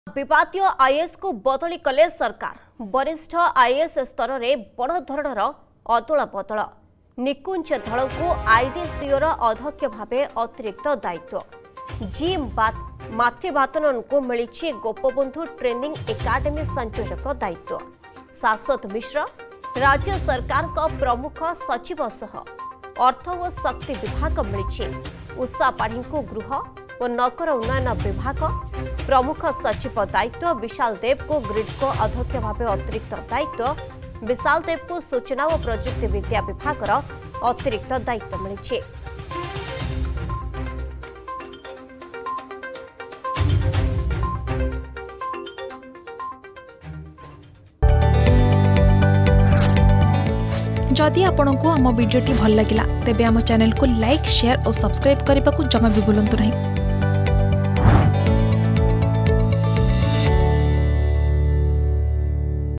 ବିବାଦୀୟ IASଙ୍କୁ ବଦଳି କଲେ ସରକାର || BJP Govt
ବିବାଦୀୟ IASଙ୍କୁ ବଦଳି କଲେ ସରକାର ।  ବରିଷ୍ଠ IAS ସ୍ତରରେ ବଡ଼ଧରଣର ଅଦଳବଦଳ । ନିକୁଞ୍ଜ ଧଳଙ୍କୁ IDCOର ଅଧ୍ୟକ୍ଷ ଭାବେ ଅତିରିକ୍ତ ଦାୟିତ୍ଵ ।  ଜି. ମାଥିଭାଥନନ୍‌ଙ୍କୁ ମିଳିଛି ଗୋପବନ୍ଧୁ ଟ୍ରେନିଂ ଏକାଡେମି ସଂଯୋଜକ ଦାୟିତ୍ଵ ।